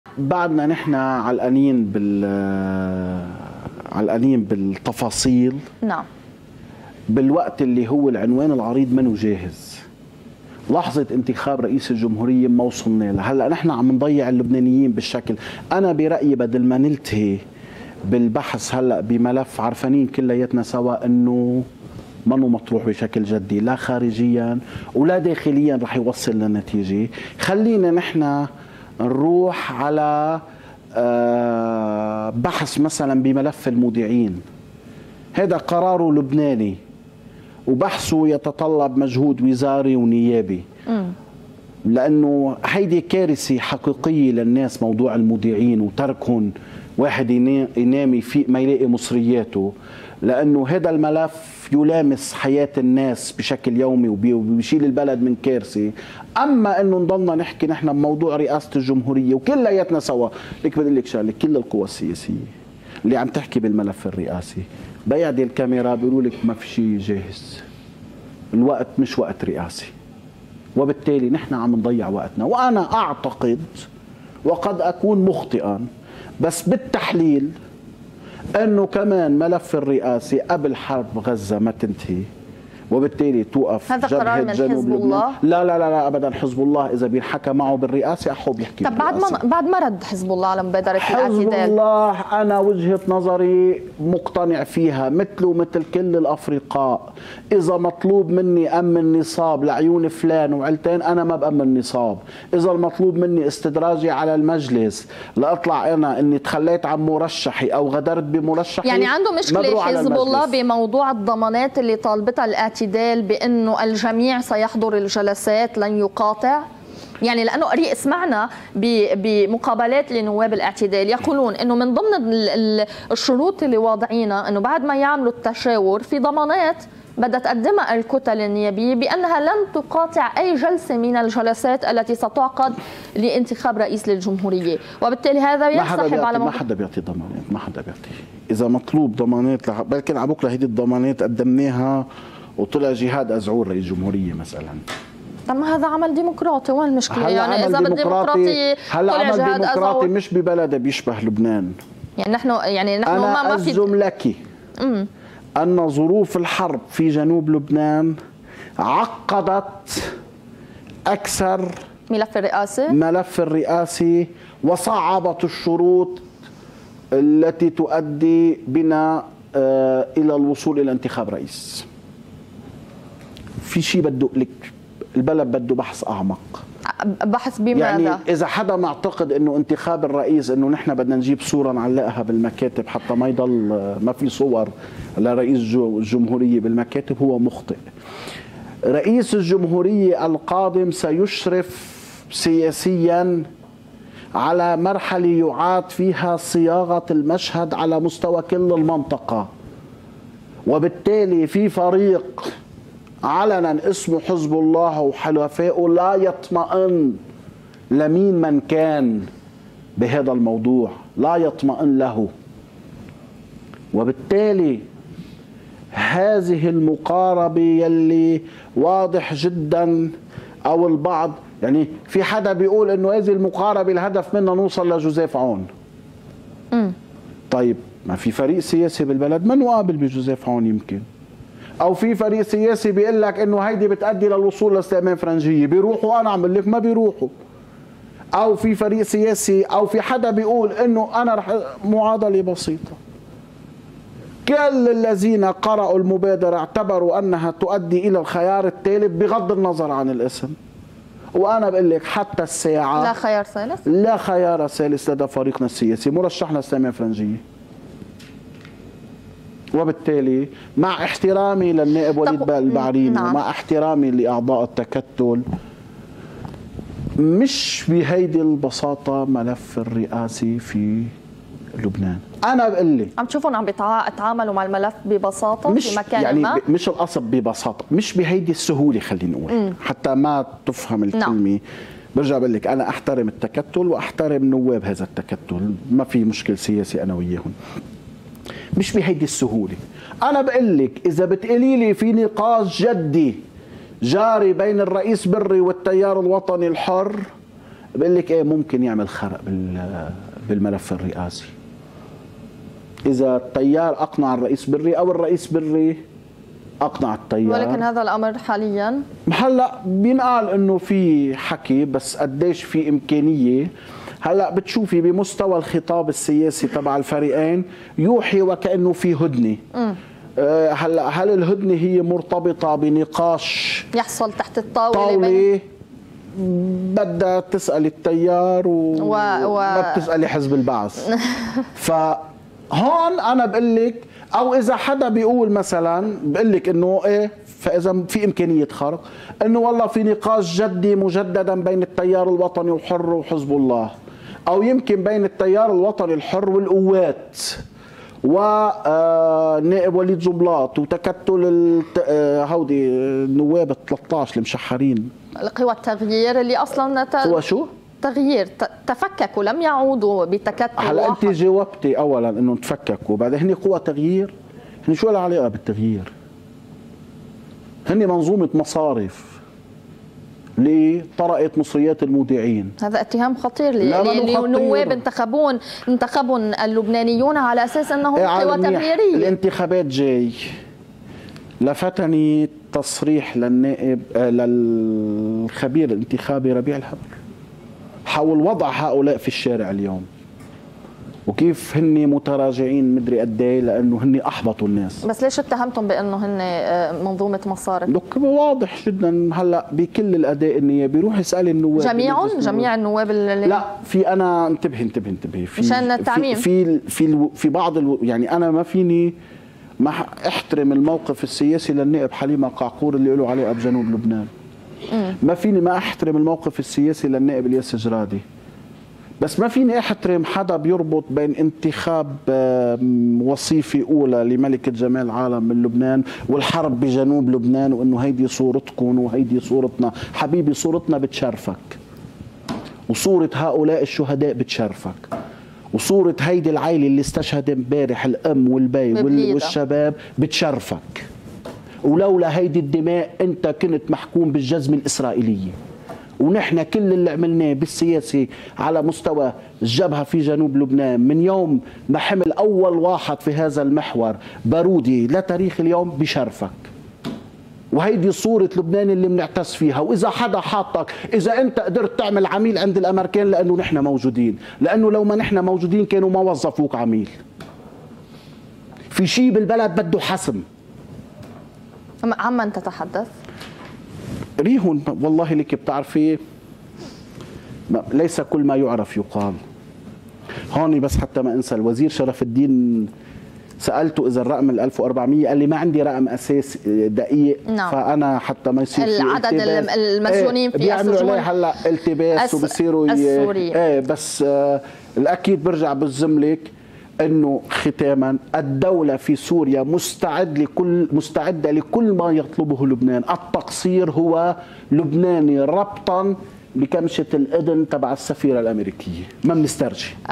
علي حجازي : لا خيار ثالث للرئاسة وفرنجية هو مرشحنا الثابت
علي حجازي